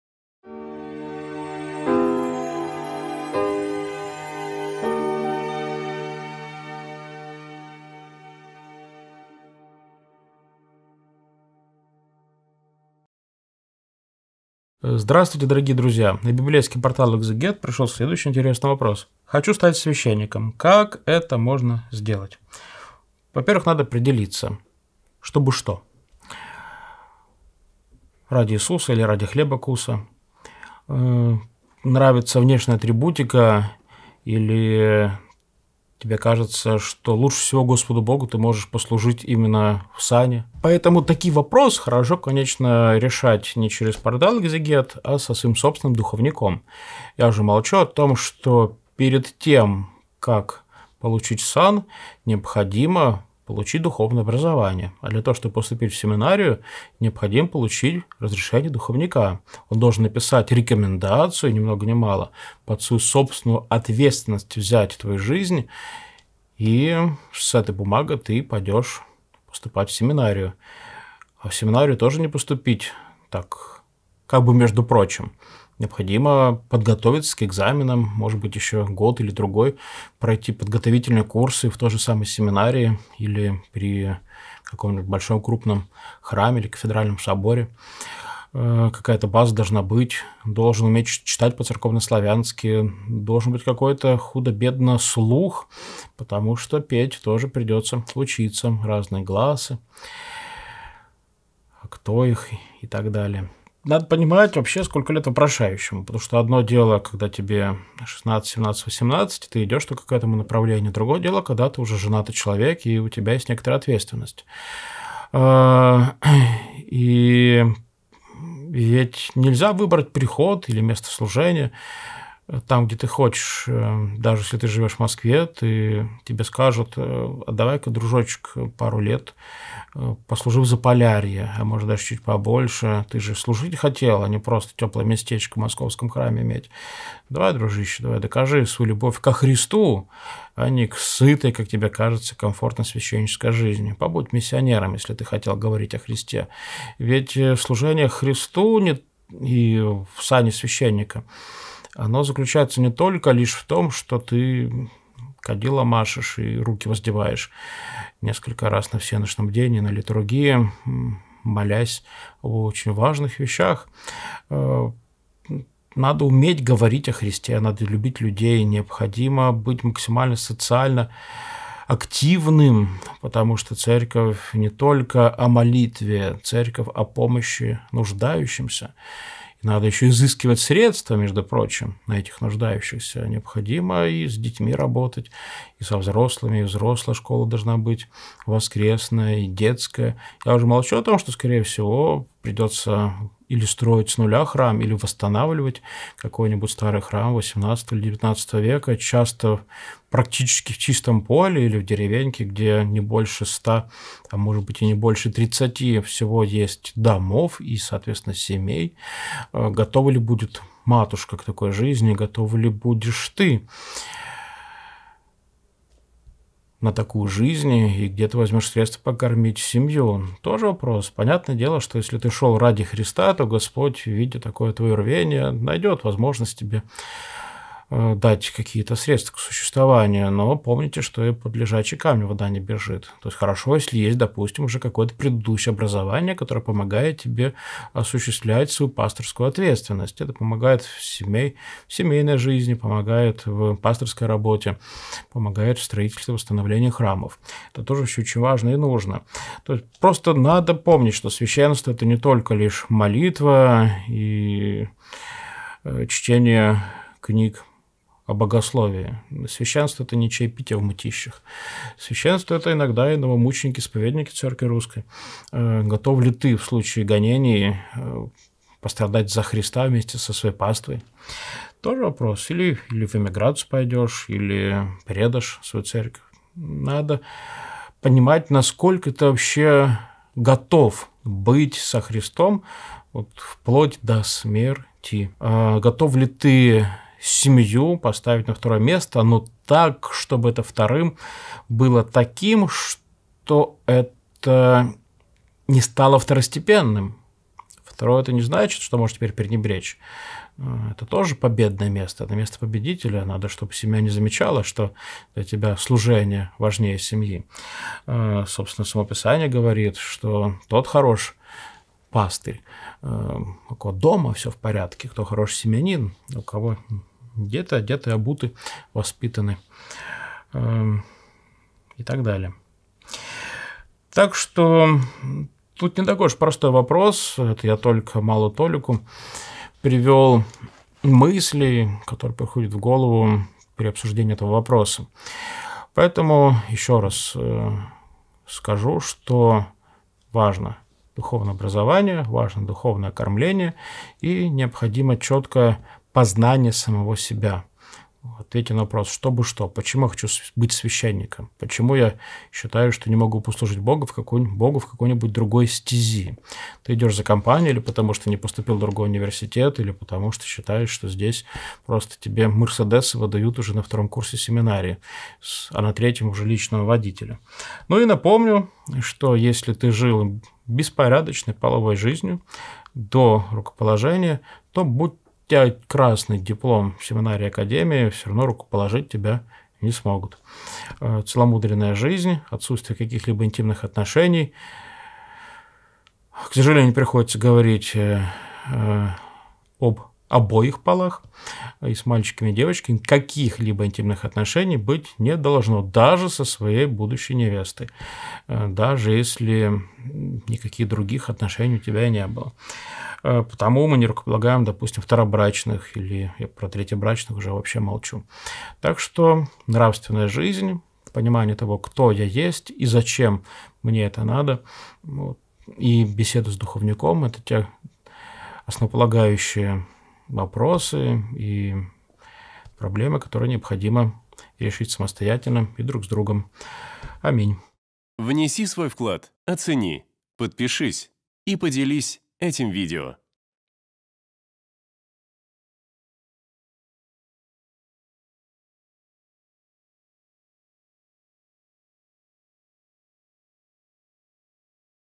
ХОЧУ СТАТЬ СВЯЩЕННИКОМ. Как это можно сделать? Священник Владислав Береговой
Команда Экзегет.

На библейском портале Экзегет священник Владислав Береговой отвечает на вопрос: "Хочу стать священником. Как это можно сделать?"

🔸  Духовное образование
🔸  Целомудрие
🔸  Благословение духовника
🔸  Жертвенное служение
🔸  Социальная активность
🔸  Готовность быть со Христом